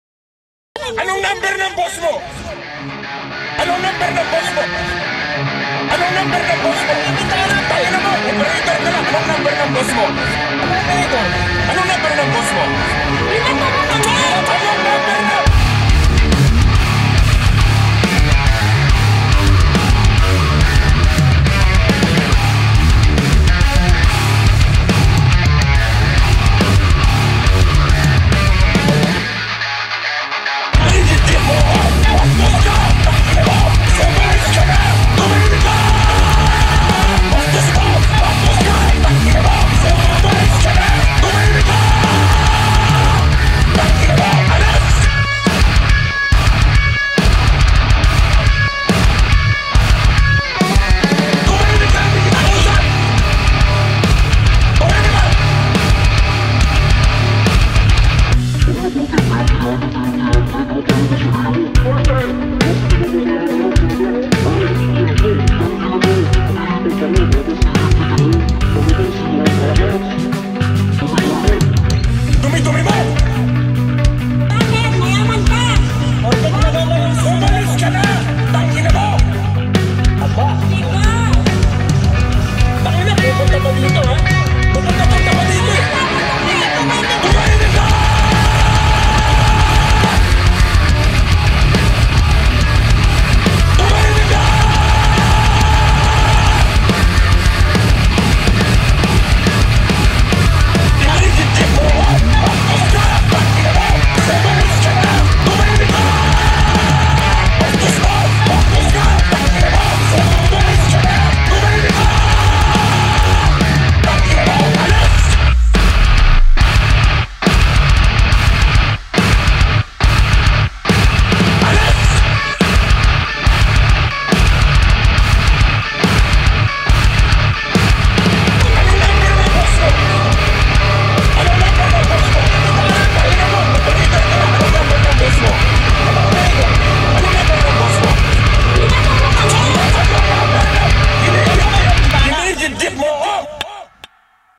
8 string metal DROP C#1
Decided to the a meme video about a local scandal.
Drop C#1 tuning

agile 830
fractal FM3
xiaomi pocophone x3 pro

composed and mixed by marween santos